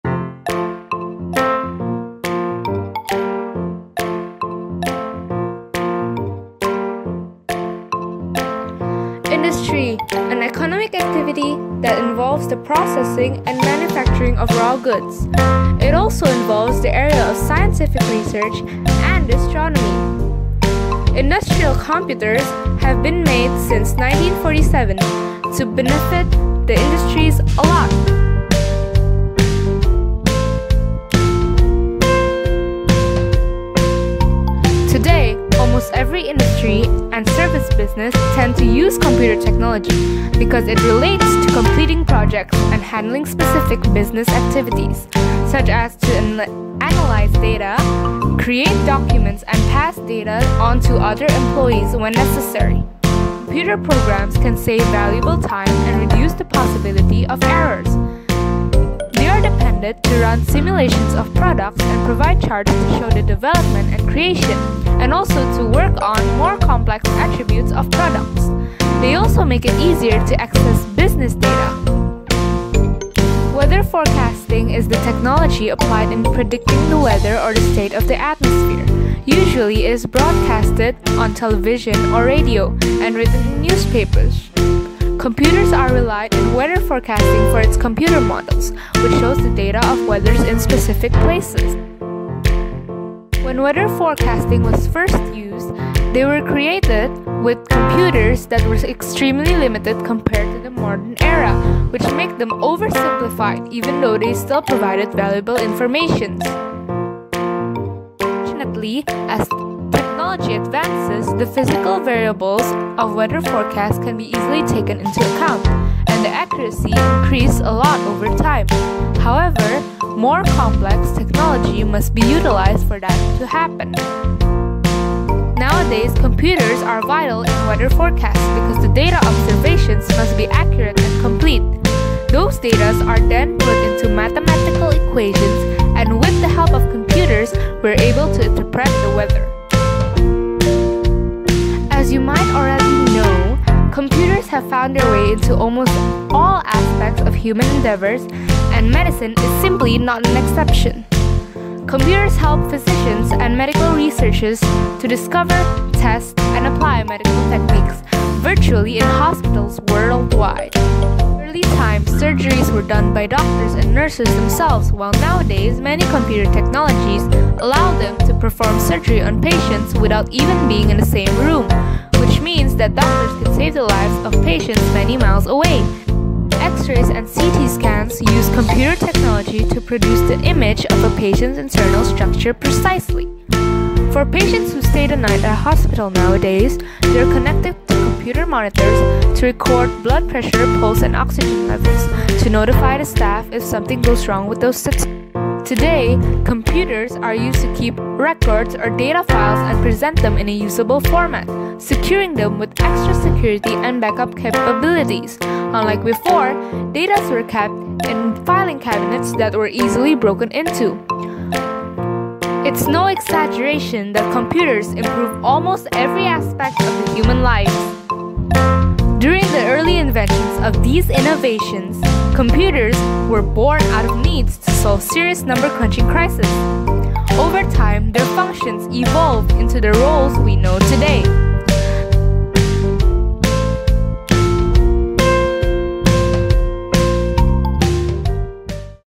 How Computers Have Changed Industries
This is a videocast project for Individuals&Societies by Giselle, Aurelia, Falco,Kezia.